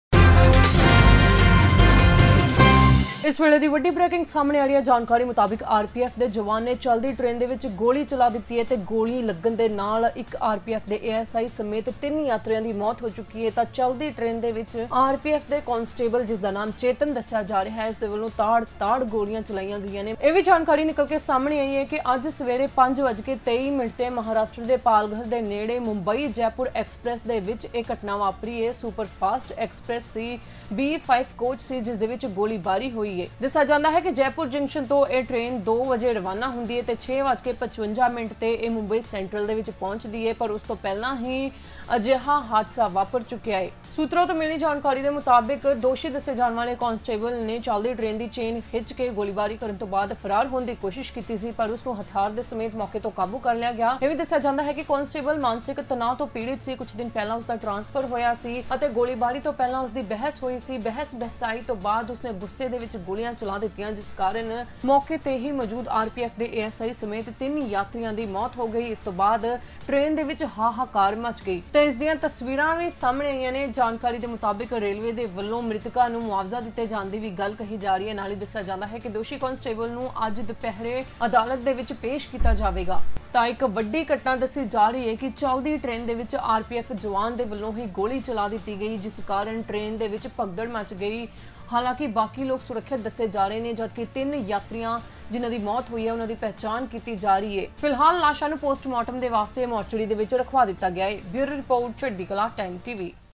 ਚਲਦੀ ਟ੍ਰੇਨ ’ਚ RPF ਦੇ ਜਵਾਨ ਨੇ ਤਾੜ-ਤਾੜ ਚਲਾਈਆਂ ਗੋਲੀਆਂ..!
*  RPF ਦੇ ASI ਸਮੇਤ 3 ਯਾਤਰੀਆਂ ਦੀ ਹੋਈ ਮੌਤ
*  ਮੁੰਬਈ-ਜੈਪੁਰ ਐਕਸਪ੍ਰੈੱਸ ’ਚ ਚੱਲੀਆਂ ਗੋਲੀਆਂ
*  ਯਾਤਰੀਆਂ ਦੀ ਕੀਤੀ ਜਾ ਰਹੀ ਪਹਿਚਾਣ
*  ਕਾਂਸਟੇਬਲ ਨੇ ਫਾਇਰਿੰਗ ਤੋਂ ਬਾਅਦ ਟ੍ਰੇਨ ਤੋਂ ਮਾਰੀ ਛਾਲ
*  ਹਥਿਆਰ ਸਮੇਤ ਕਾਂਸਟੇਬਲ ਨੂੰ ਕੀਤਾ ਕਾਬੂ
*  ਮਾਨਸਿਕ ਤੌਰ ’ਤੇ ਪਰੇਸ਼ਾਨ ਸੀ ਕਾਂਸਟੇਬਲ- ਸੂਤਰ
*  ਬਹਿਸ ਤੋਂ ਬਾਅਦ ਚਲਾਈਆਂ ਗੋਲੀਆਂ- ਸੂਤਰ

* 3 passengers including ASI of RPF died
* Bullets fired in Mumbai-Jaipur Express
* Identification of passengers
* The constable jumped from the train after firing
* Constable arrested with weapon
* The constable was mentally disturbed - Sutra
* Shots fired after the argument - Sutra